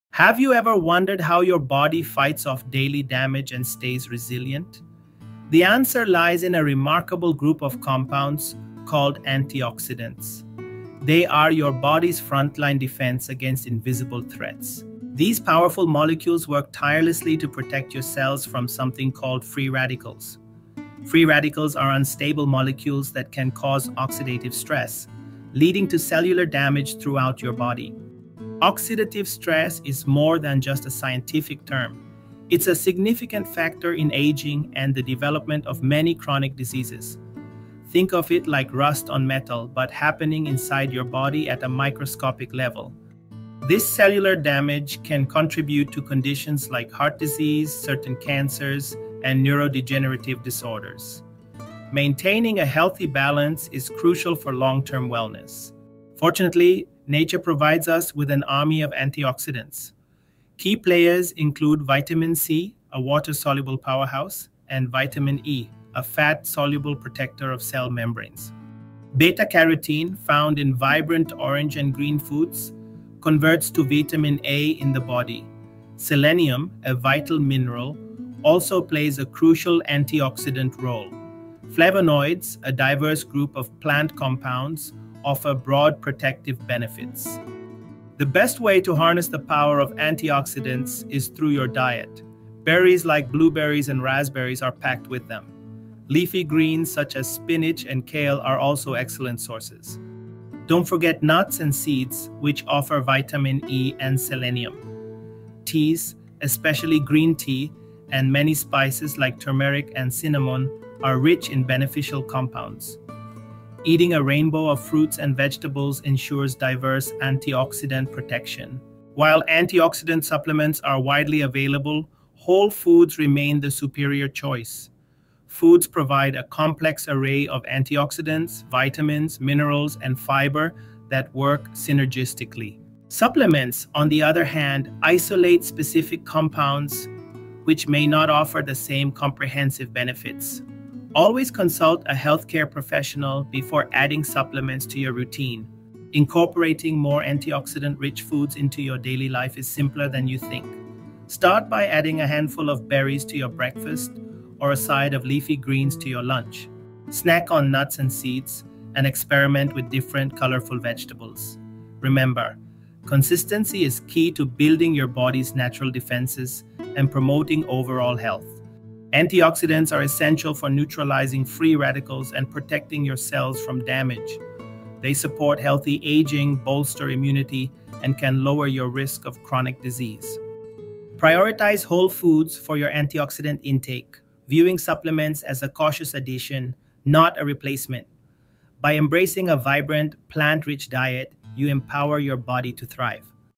Understanding the Role of Antioxidants—Protecting Your Cells & Boosting Health
Antioxidants are your body’s defense against free radicals — unstable molecules that cause oxidative stress and damage cells. In this video, we explore the science behind antioxidants, their role in disease prevention, and how to get them naturally through diet.

🔍 What You’ll Learn:
- What antioxidants are and how they work in the body
- The link between oxidative stress, aging, and chronic disease
- Common antioxidants: Vitamin C, Vitamin E, beta‑carotene, selenium, flavonoids
- Best food sources: berries, leafy greens, nuts, seeds, teas, and spices
- The difference between whole‑food antioxidants and supplements
- Practical tips to boost antioxidant intake safely

✅ Quick Takeaways:
- Antioxidants neutralize free radicals, protecting cells from damage
- They support healthy aging, immunity, and lower risk of chronic disease
- Whole foods are the best source — supplements should be used cautiously
- Eating a rainbow of fruits and vegetables ensures diverse antioxidant protection

📚 References
- Harvard Nutrition Source – Antioxidants
- WebMD – Health Benefits of Antioxidants
- Cleveland Clinic – What Do Antioxidants Do
- Healthline – Antioxidants Explained
- Medical News Today – Antioxidants

Pinned Comment:
Which antioxidant‑rich food do you eat most often — berries, greens, or nuts? Share below.